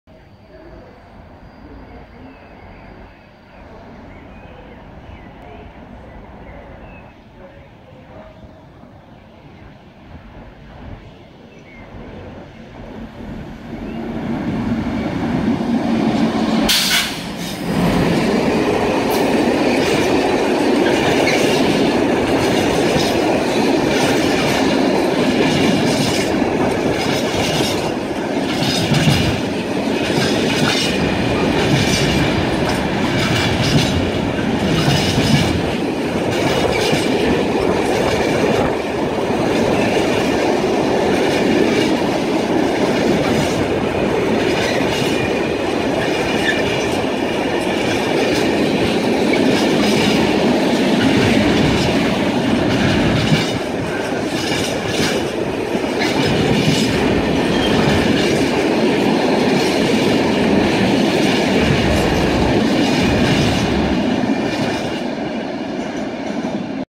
Colas 60002 passes through Oxford
A quick video of Colas 60002 passing through Oxford on 14.18 Colnbrook - Lindsey Refinery on Monday 28th March 2016. Enjoy!